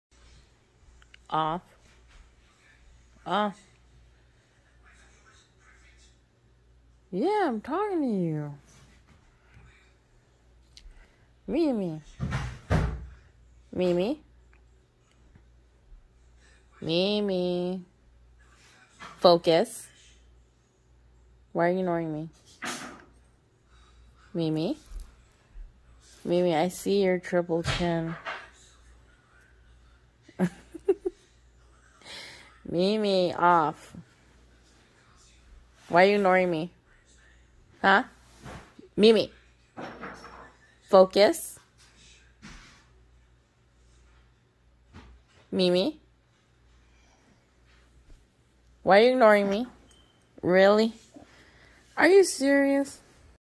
Bullmastiff Puppy: Ultimate Ignore Challenge!
The ultimate ignore challenge from Mimi the Bullmastiff. Gotta love this breed 😆